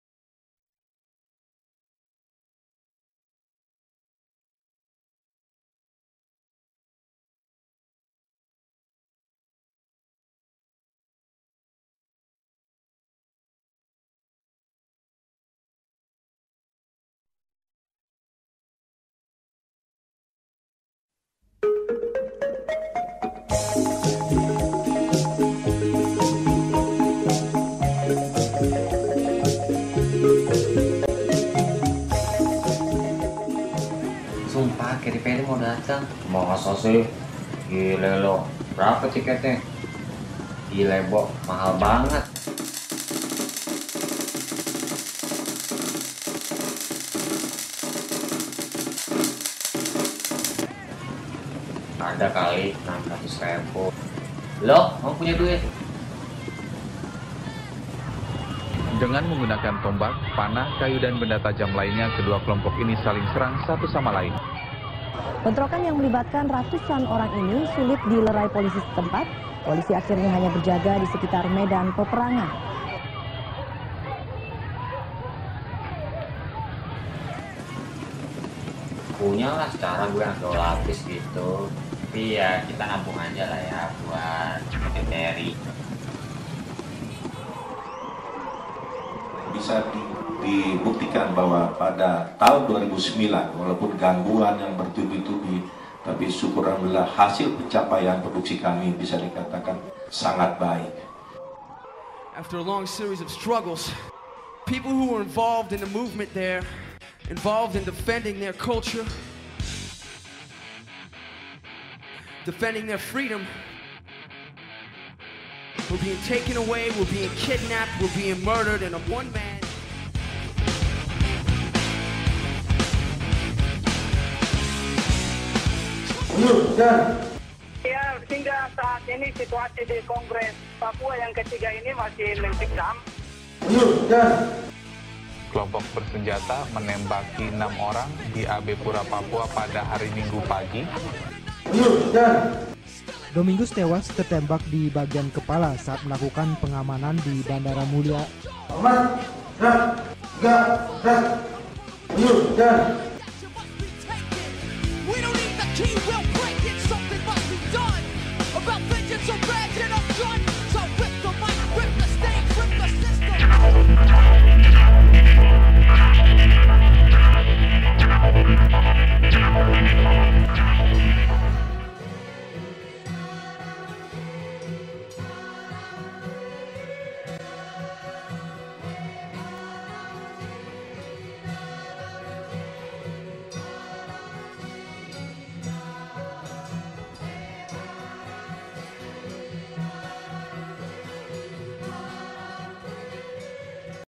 VIDEO REMIND - 08 - ABU-ABU DI POJOK TIMUR
Video Remind merupakan hasil karya dari peserta workshop yang diadakan oleh KontraS dan Ruang Rupa. hal ini dimaksudkan untuk menjembatani komunikasi yang hilang antara orang papua dengan masyarakat Indonesia lainnya, antara korban dengan anak-anak muda saat ini. dan disini mencoba untuk partisipan berlaku secara kreatif terhadap dokumen-dokumen KontraS sejak berdirinya